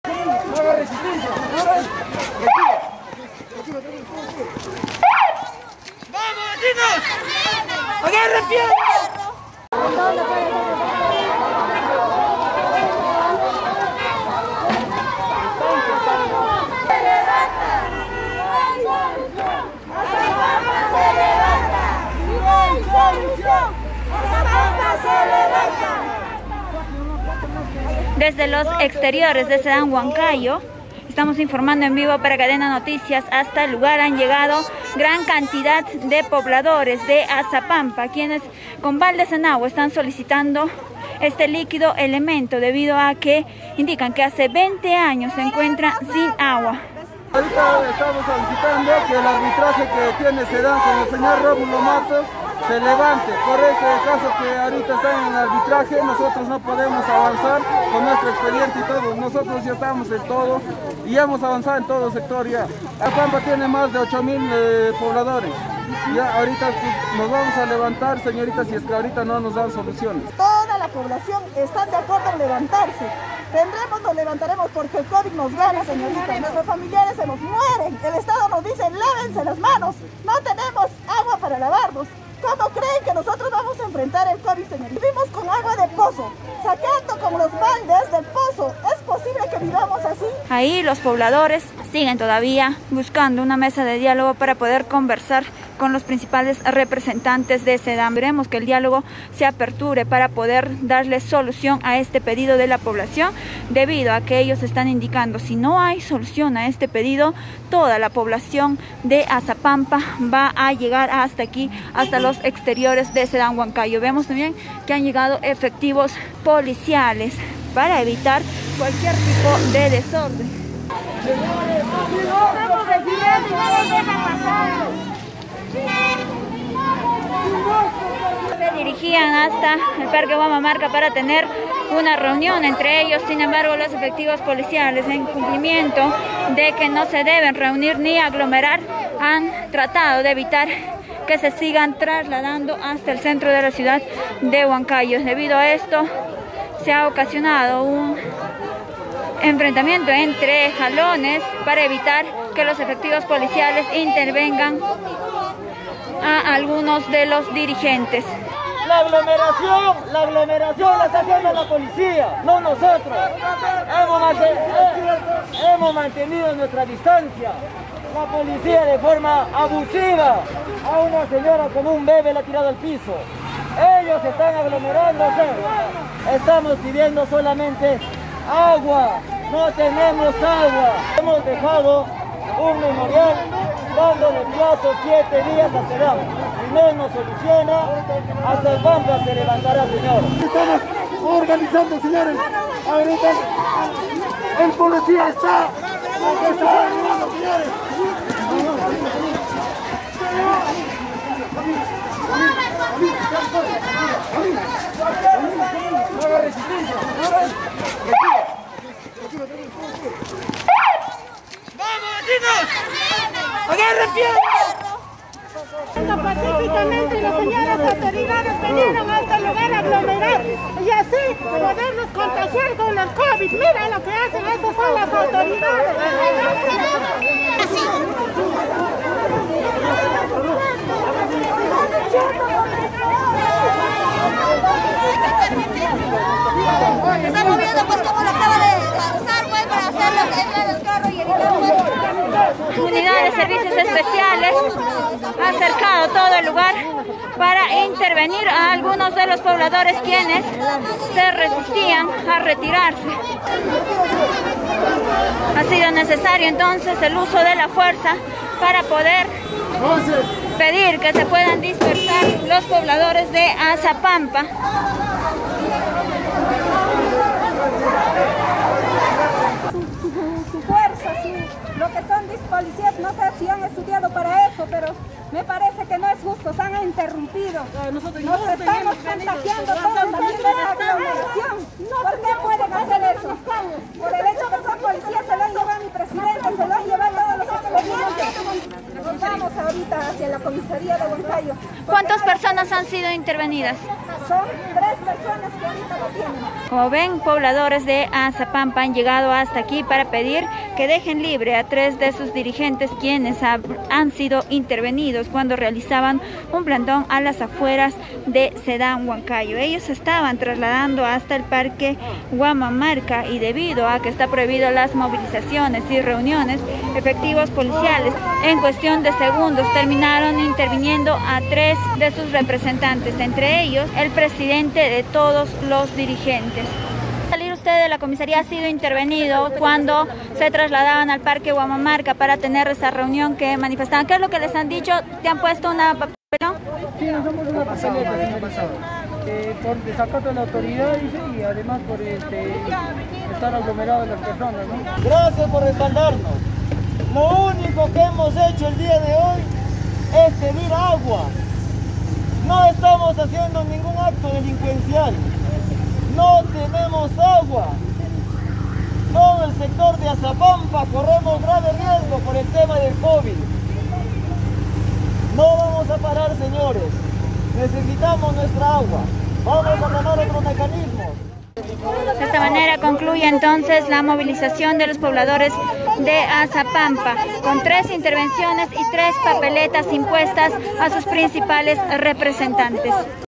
HUANCAYO: Enfrenamiento en protesta por agua | KDNA NOTICIAS
Pobladores del sector de Azapampa protestaron en los exteriores de SEDAM, ellos pedían se les brinde mayor atención en su problema que es la falta de agua. Debido a que se durante la protesta hubieron enfrentamientos, la PNP detuvo a tres pobladores.